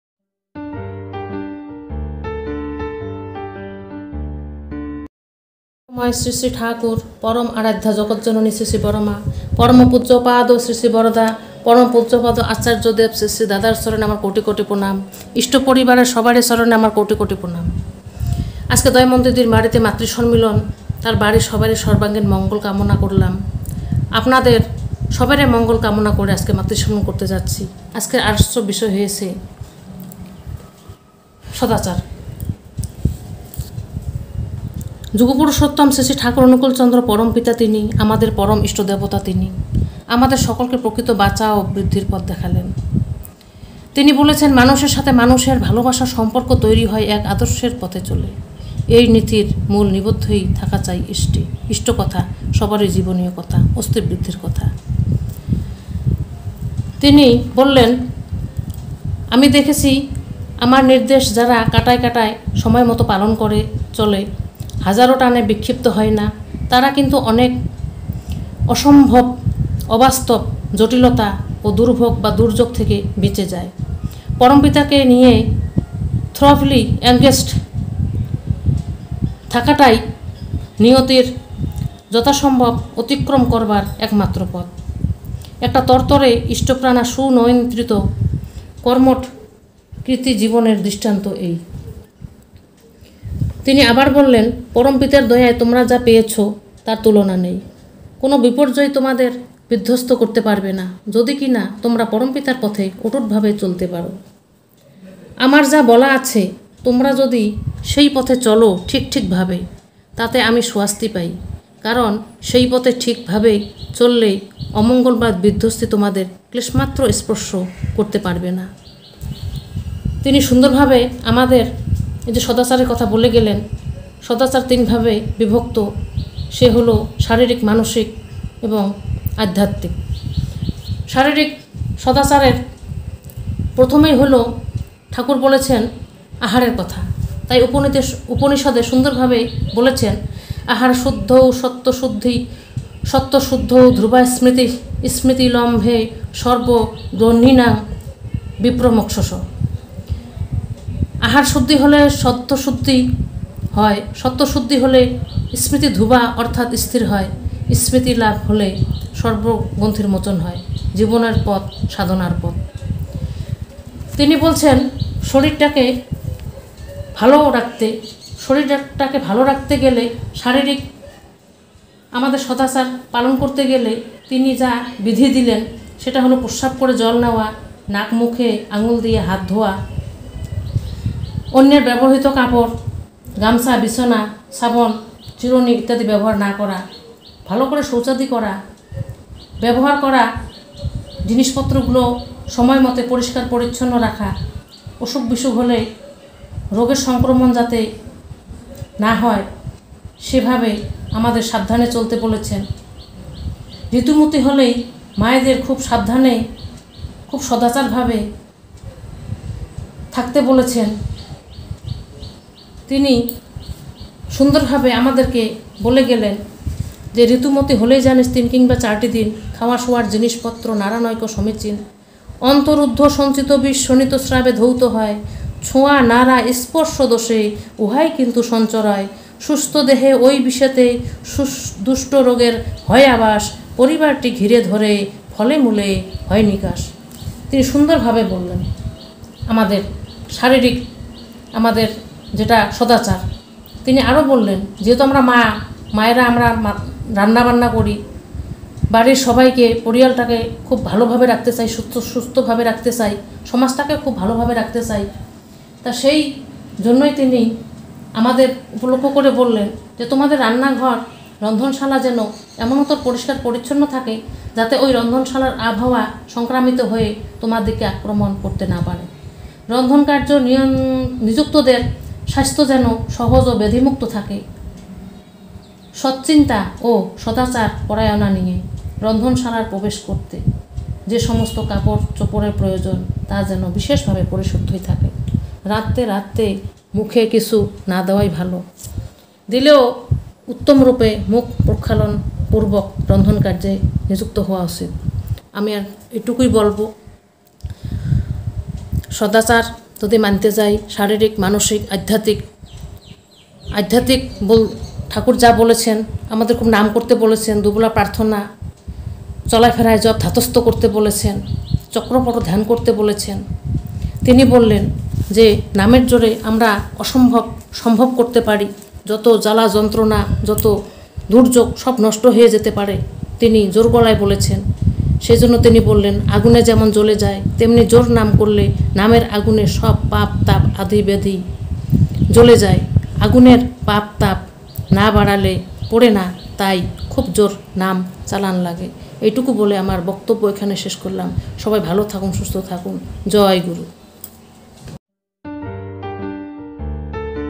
Alochona// bisoy:-Sadachar
Istoprosongo.
 Alochona korben Srimoti Gayetri Ray Maa (Gossaigoan Assam.)
 bisoy-Sadachar.